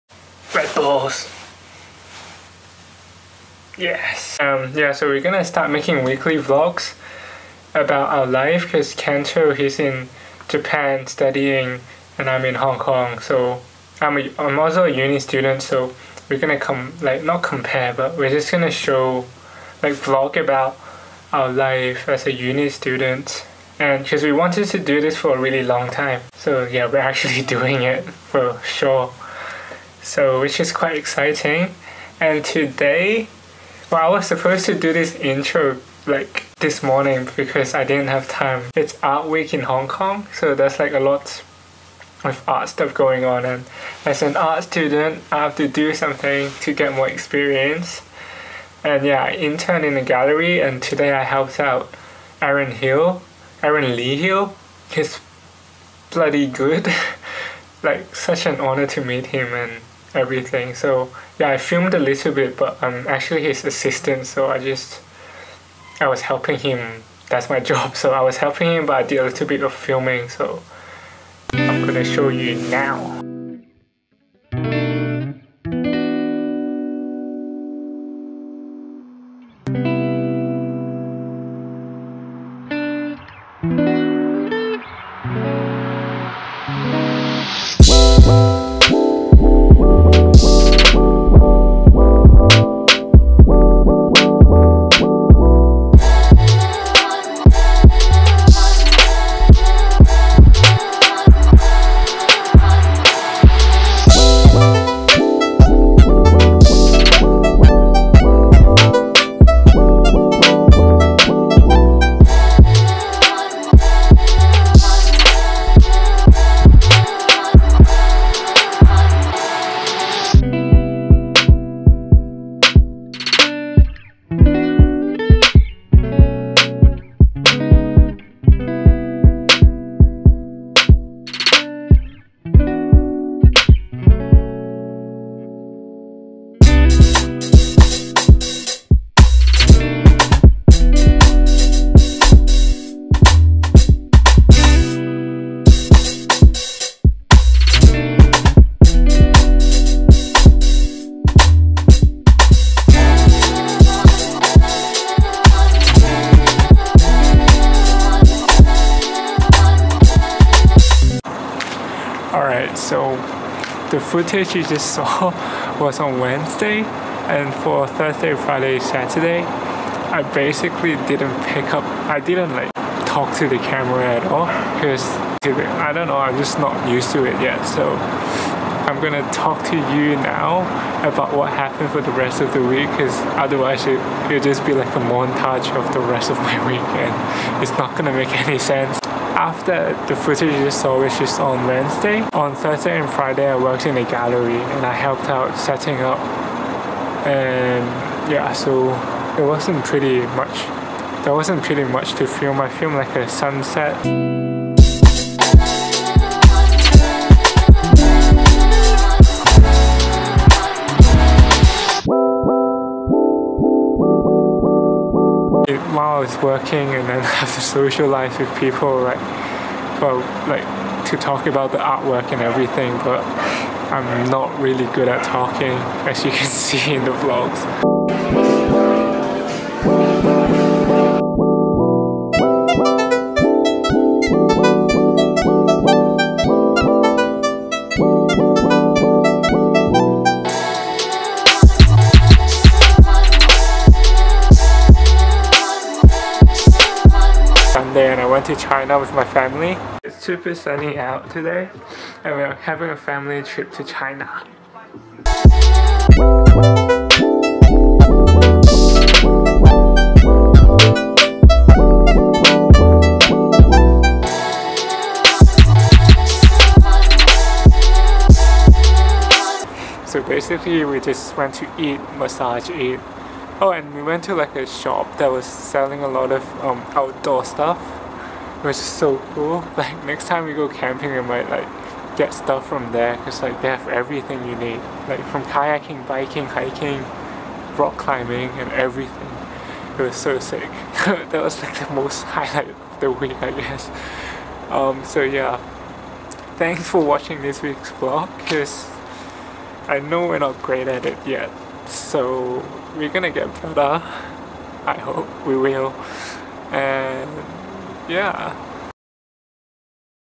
First Week of Weekly Vlog - Art Week in Hong Kong
This is our first week of weekly vlog, where we show you our highlights of our week and what we are up to, hope you guys will like it- E